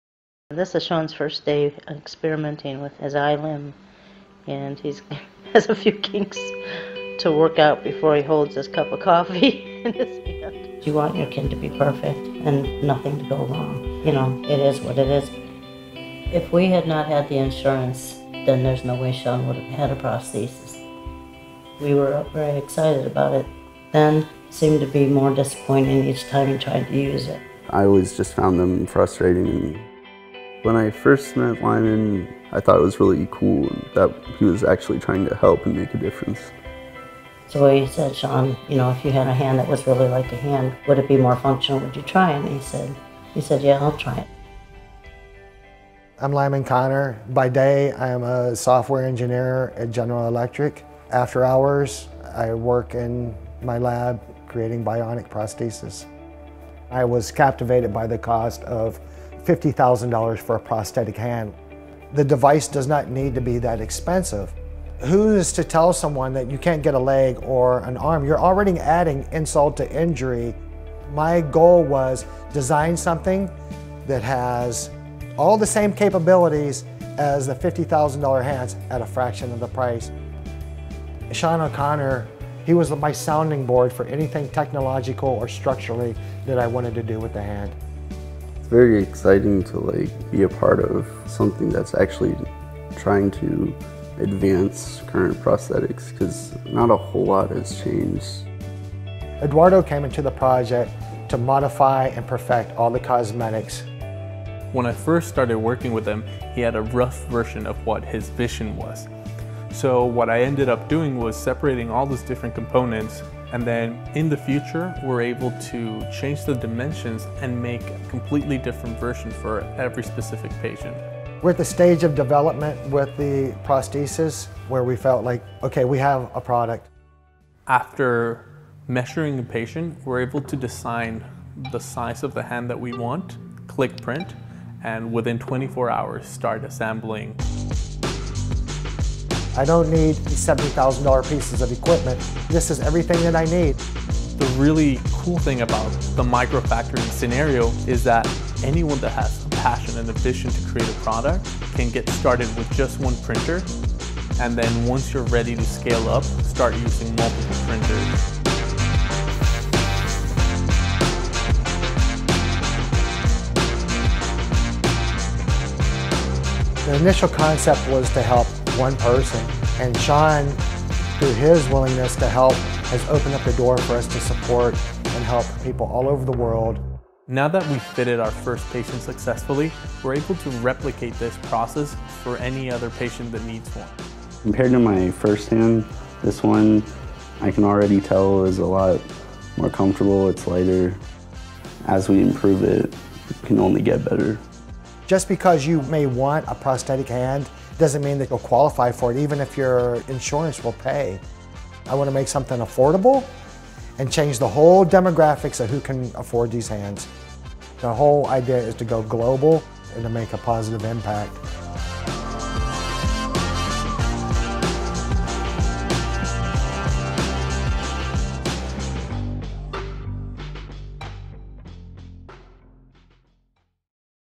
Formlabs adds new ceramic resins and tools for experimenters
3 D printers from Formlabs have gotten better .With improved and reformulated materials including a new gray and a ceramic material , Formlabs 3D printers make objects look even more solid and smooth.Also, they’ve launched Form X, an experimental toolkit for makers and engineers.